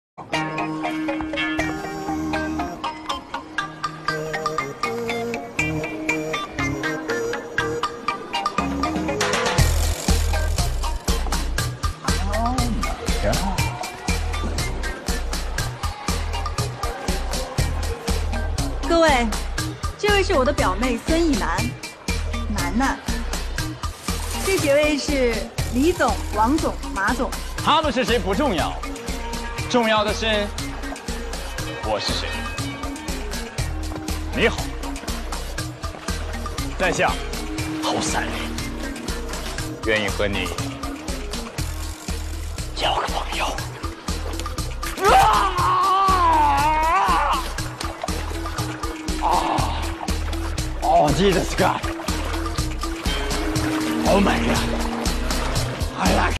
beauty
The female president took her sister to see the customer. It was too beautiful at the moment of the show, and the customer was not calm.